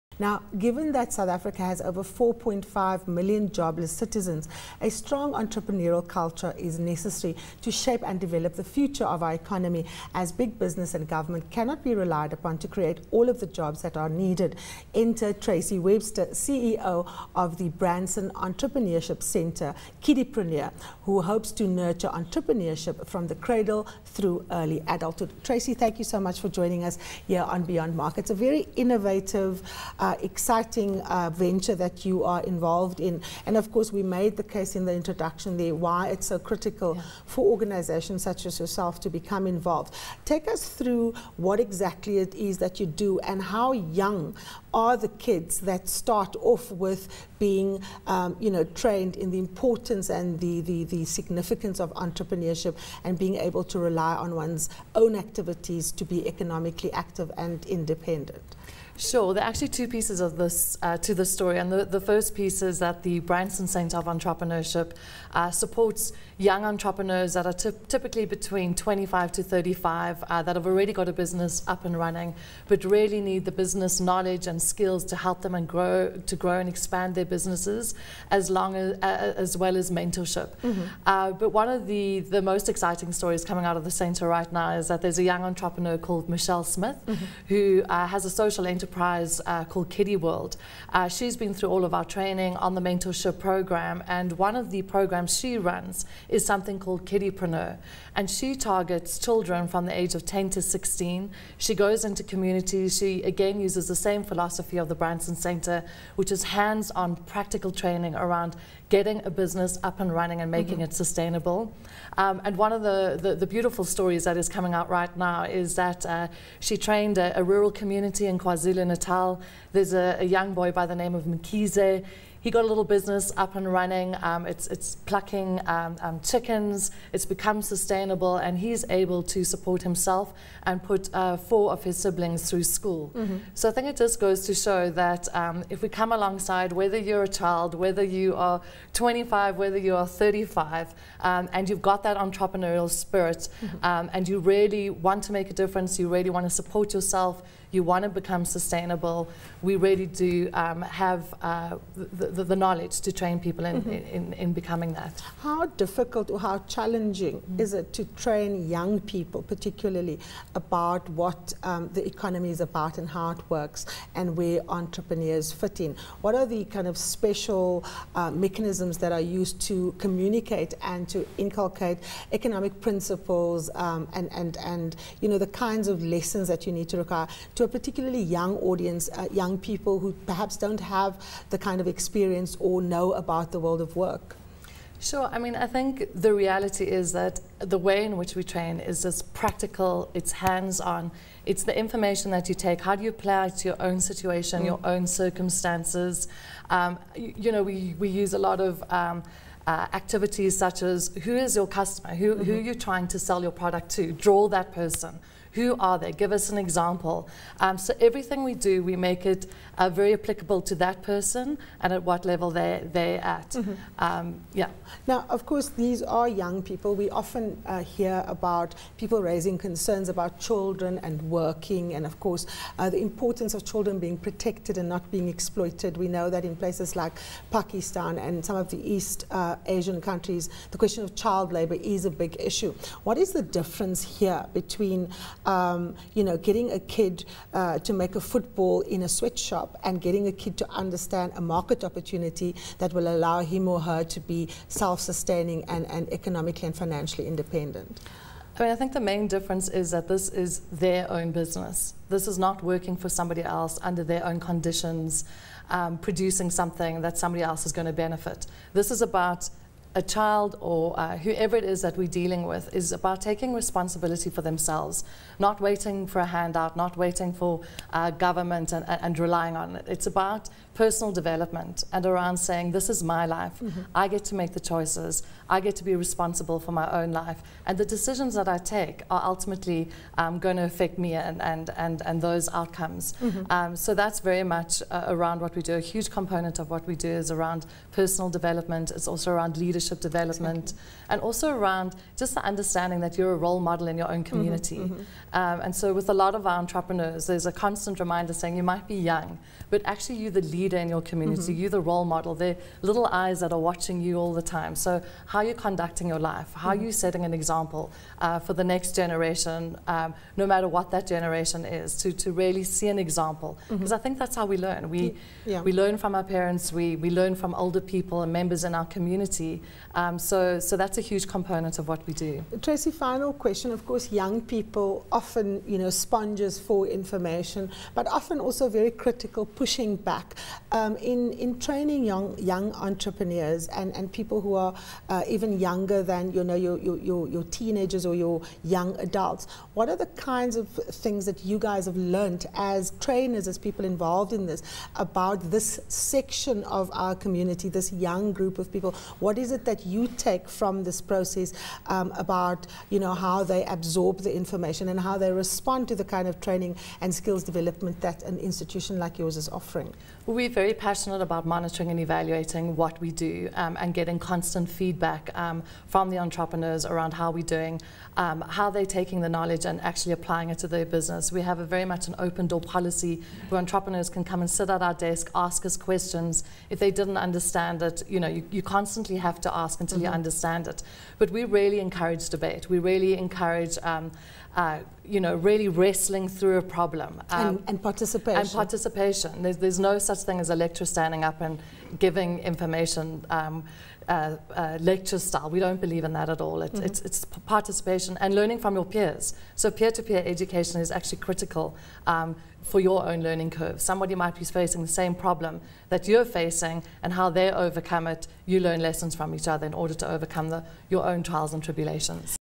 Nurturing Entrepreneurship from the Cradle
Given that South Africa has over 4.5 million jobless citizens, a strong entrepreneurial culture is necessary to shape and develop the future of our economy.